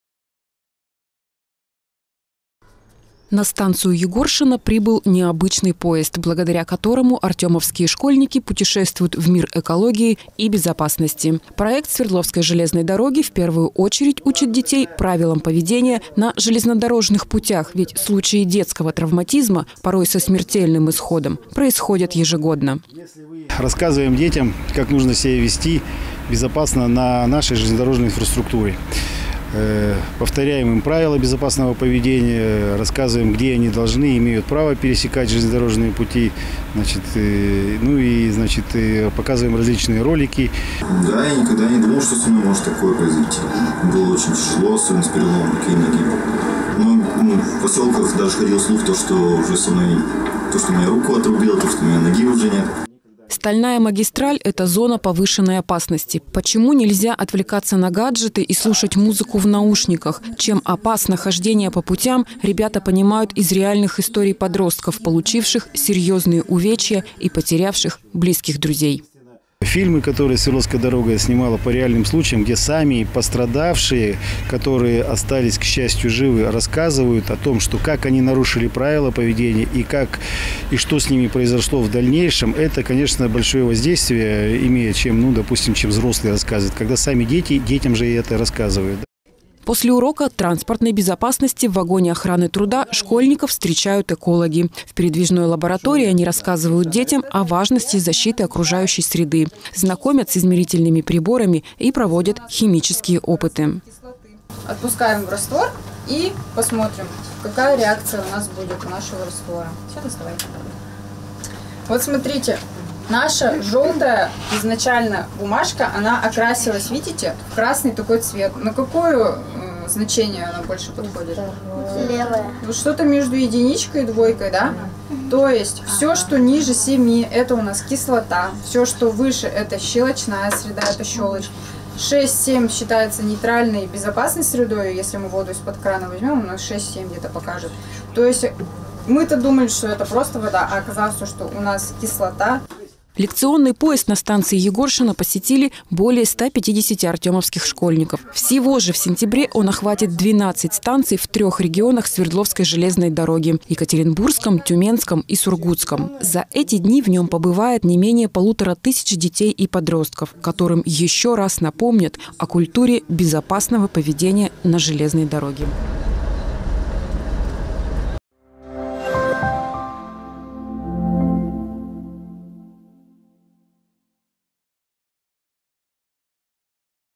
"Поезд знаний" прибыл на станцию Егоршино
На станцию Егоршино прибыл необычный поезд, благодаря которому артёмовские школьники путешествуют в мир экологии и безопасности.